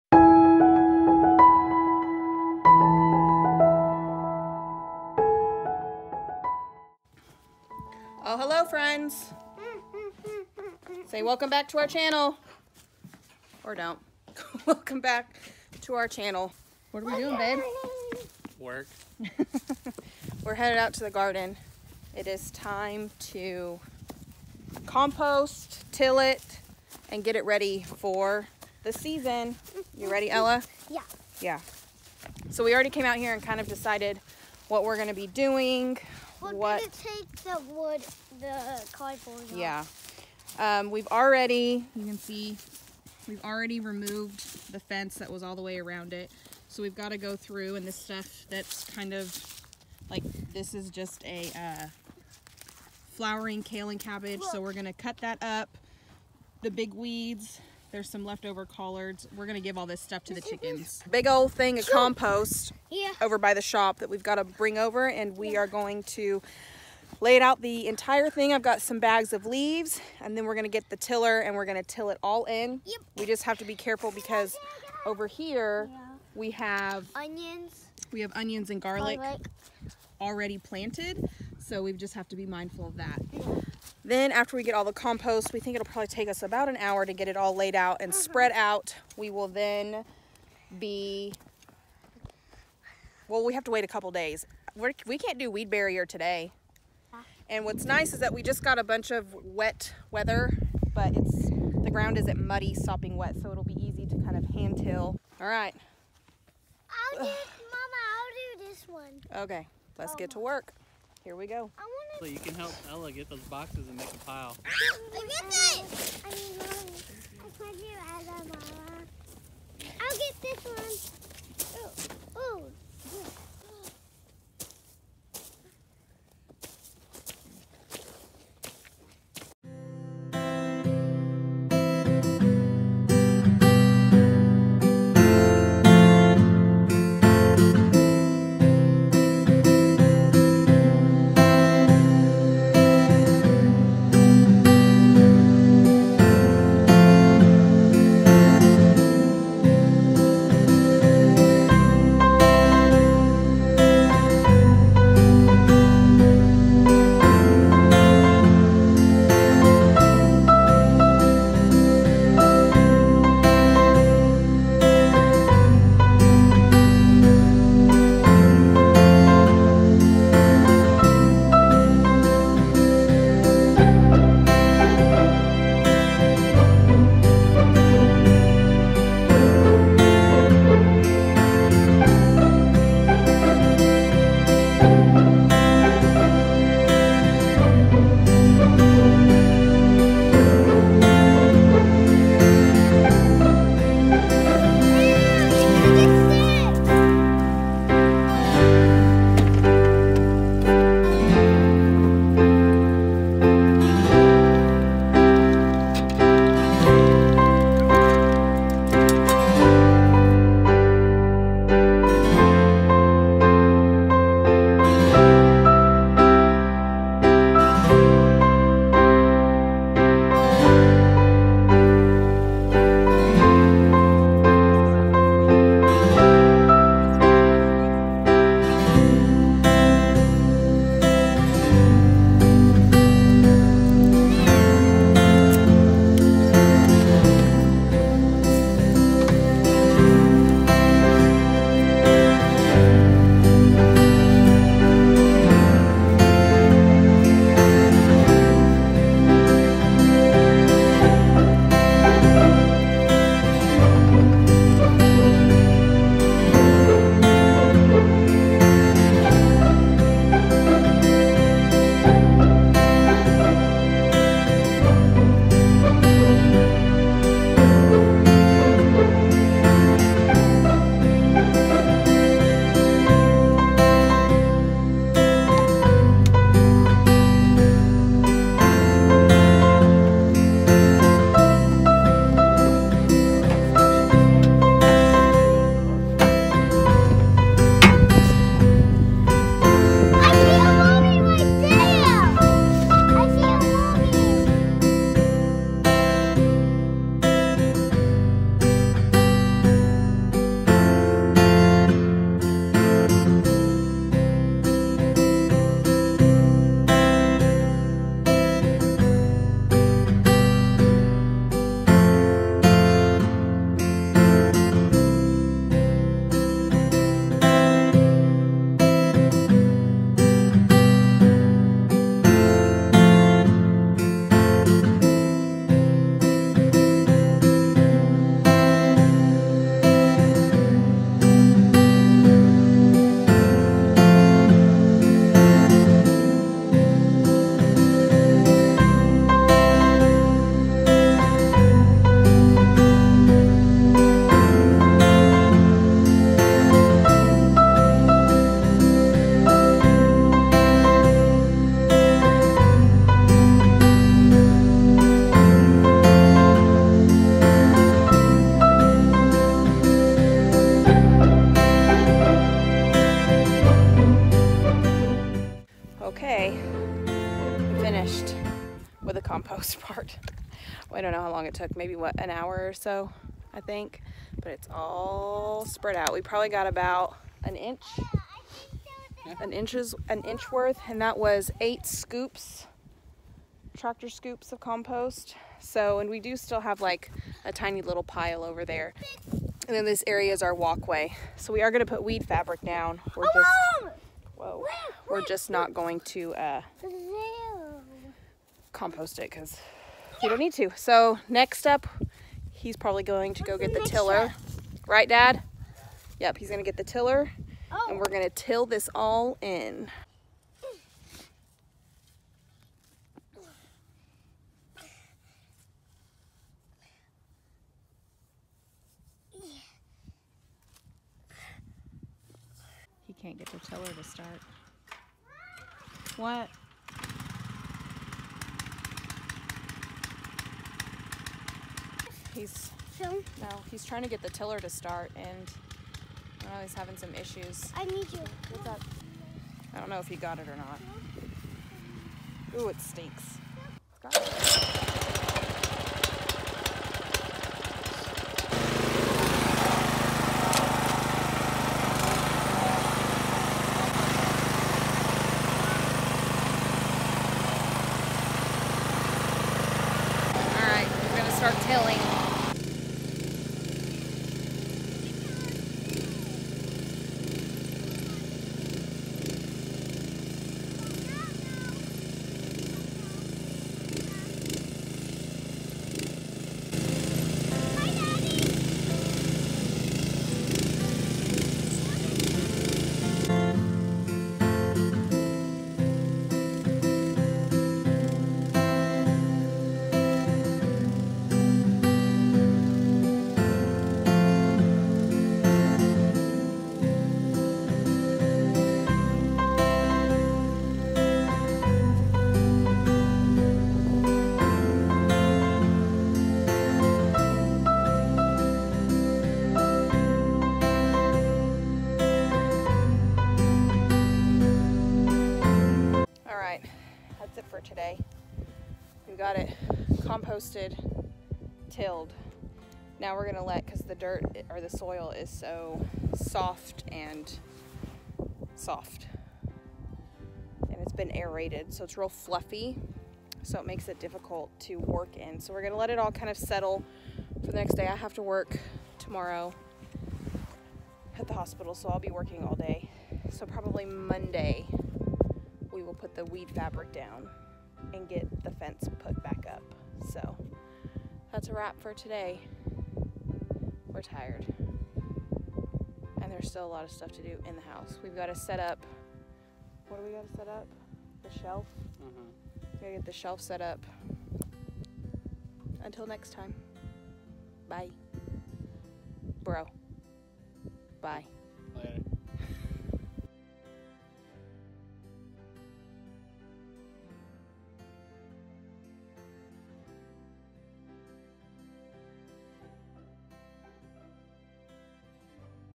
Grab a SHOVEL and let's COMPOST | 2021 Spring Garden PREP | VLOG
Time to shovel all that compost and till the garden in.